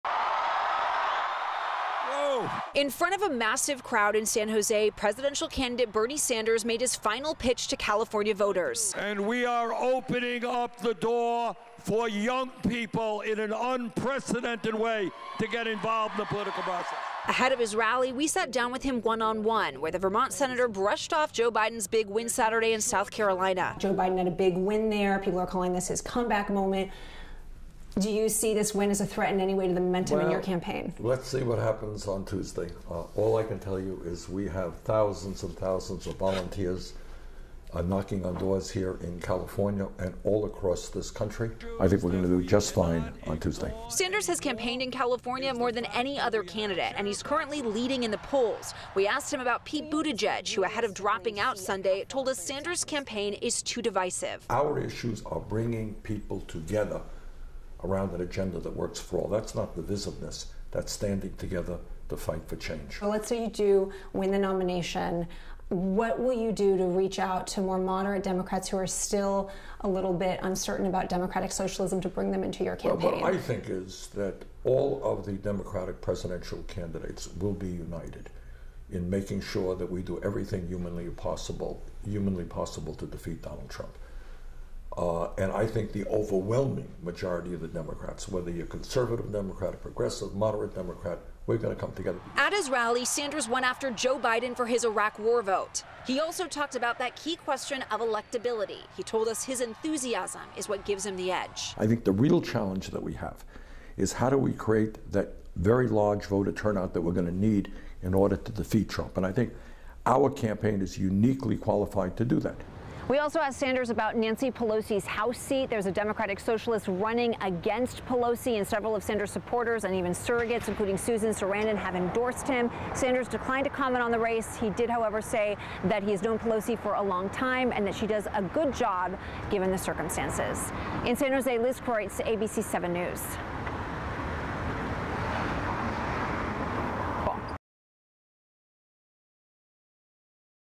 Bernie Sanders Visits Bay Area Before Super Tuesday 3-1-20
Vermont Senator, and Democratic presidential candidate Bernie Sanders visited San Jose to shore up support before Super Tuesday. Liz Kruetz reports.